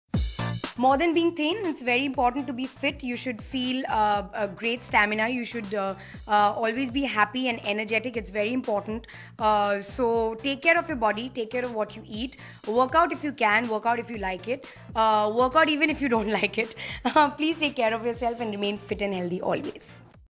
Parineeti Chopra: Workout even if you don't like it
Actor Parineeti talks about the importance of workout and staying healthy. She appeal for you to stay take up regular exercise. The NDTV-Fortis Health4U campaign does just that, encourages people to take control of their health and promote healthy living as a way of life.